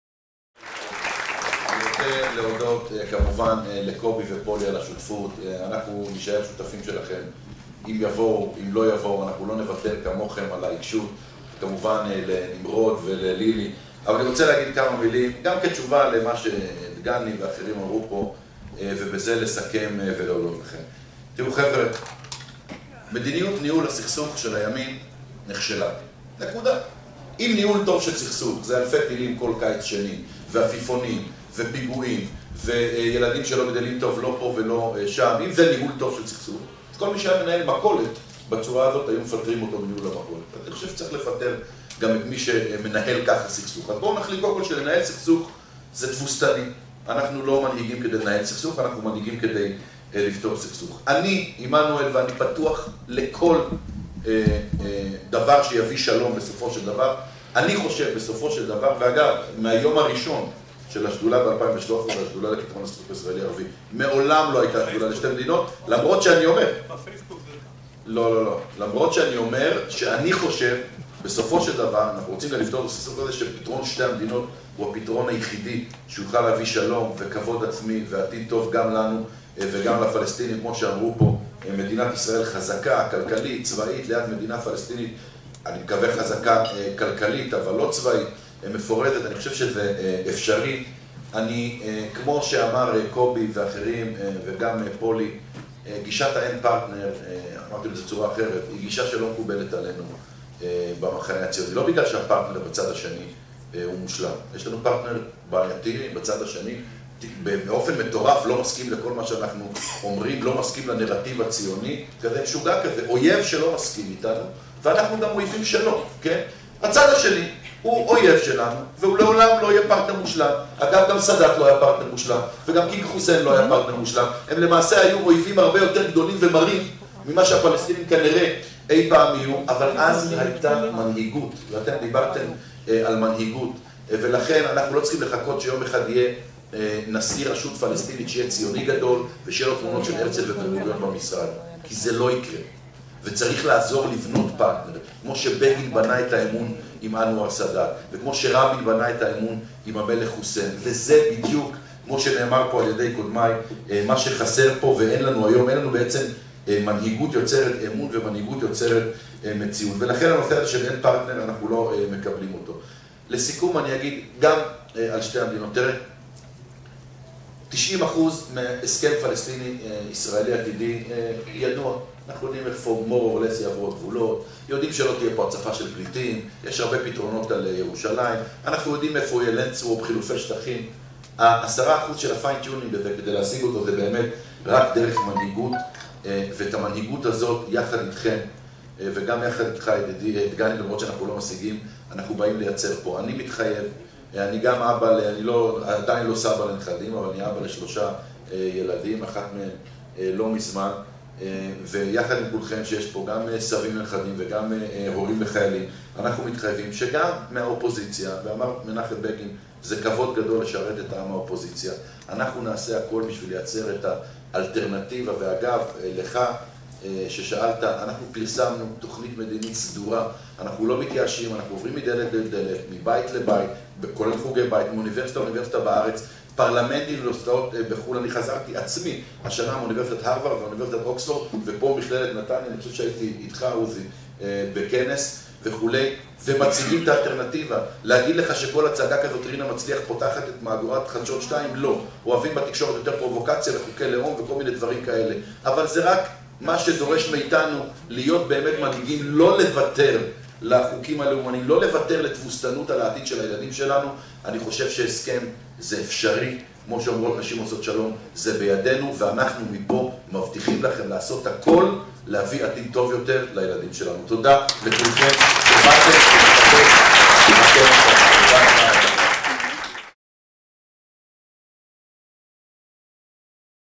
ח"כ חיליק בר - השדולה לפתרון הסכסוך הישראלי-ערבי 2018
ב-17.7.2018 עלינו לכנסת ישראל יחד עם כ-170 איש ואישה לכינוס השדולה בראשות ח״כ חיליק בר (המחנה הציוני, יו"ר השדולה) ואלו הדברים שנשא בכנס.